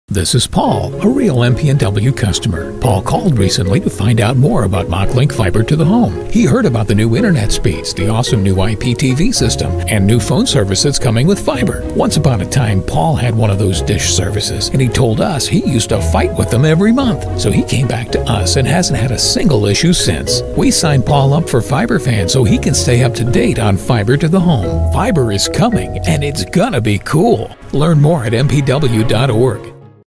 Be A FIBER FAN, like Paul!
One of our customers is a real FIBER FAN and he wants you to know why!